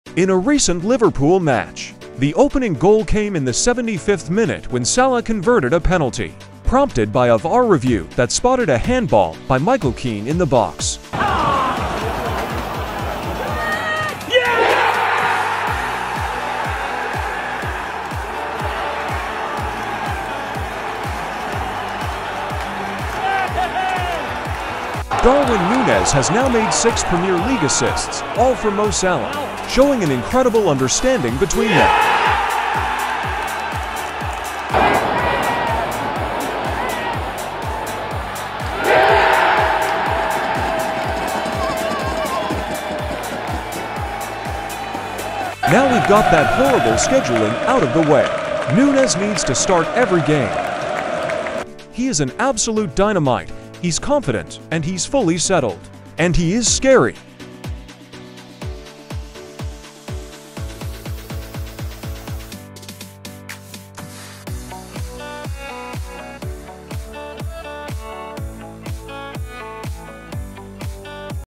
Mo Salah TWO GOALs vs EVERTON | Darwin Nunez Assist
Liverpool vs Everton 2-0  Goals and Highlights | Mo Salah 2 Goals | Darwin Nunez Assist